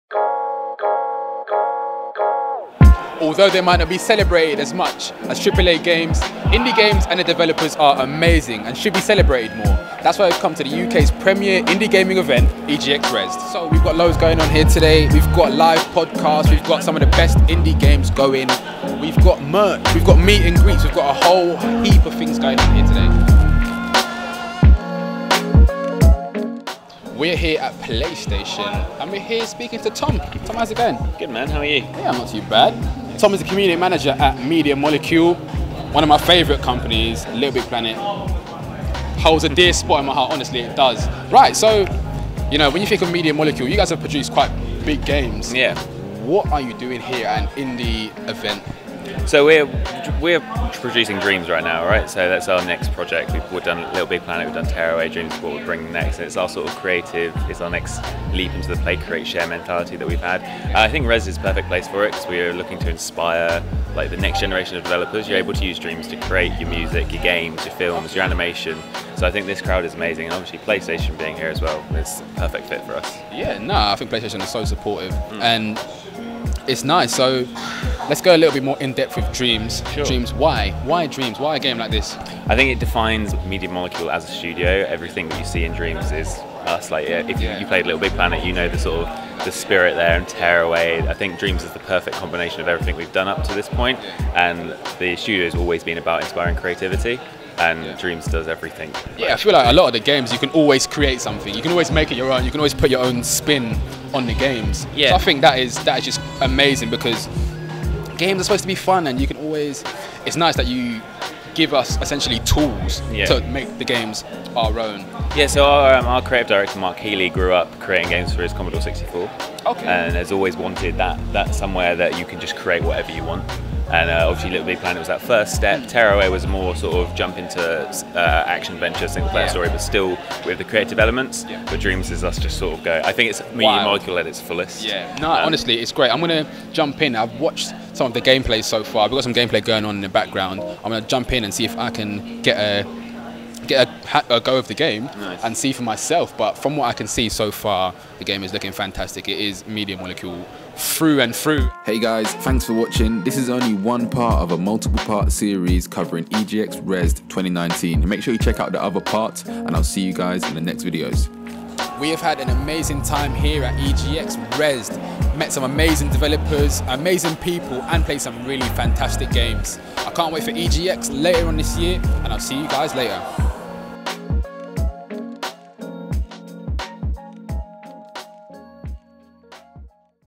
Media Molecule's Tom Dent interview | EGX REZZED 2019 Part 2
This is the second part of my EGX coverage where I head over to the PlayStation area and had the pleasure of speaking with Media Molecules community manager Tom Dent about their new game Dreams and why a big dev team like MM is at an indie games event.